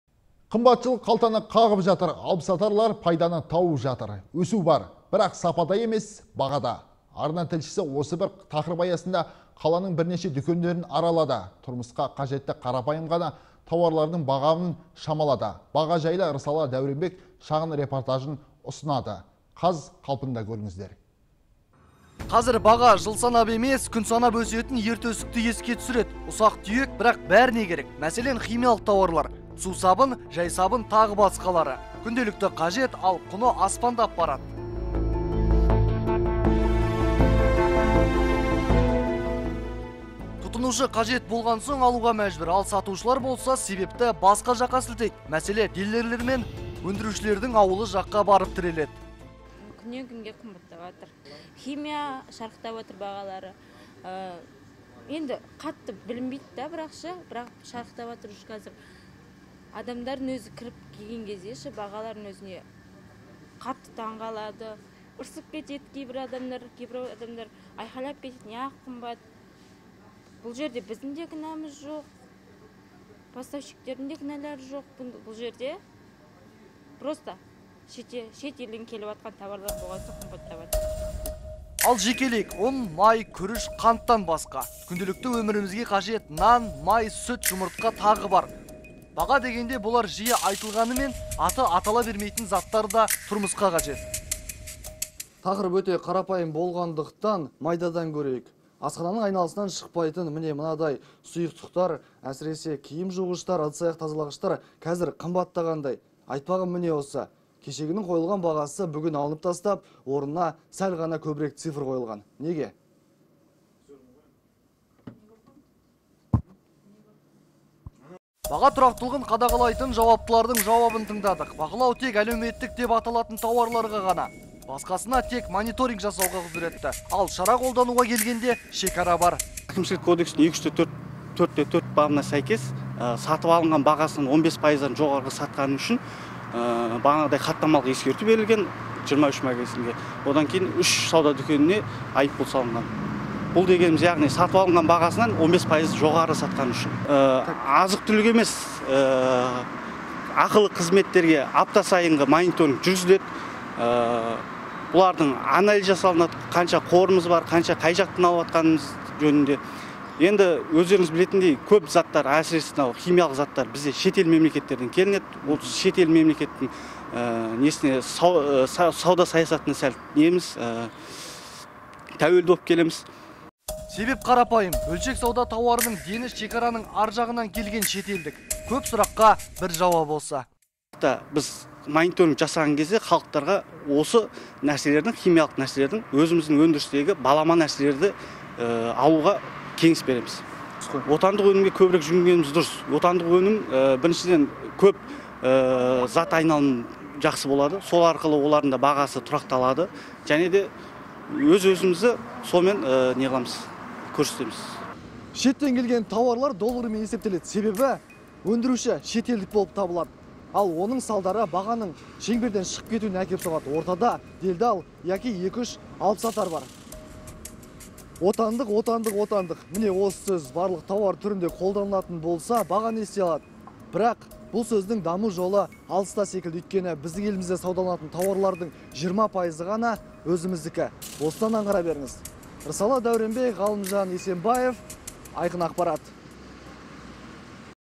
Бағада тұрақ бар ма?
Қымбатшылық қалтаны қағып жатыр, алыпсатар пайданы тауып жатыр. Өсу бар! Бірақ сапада емес, бағада. Тұрмысқа қажетті қарапайым ғана тауарлардың бағамын шамалады.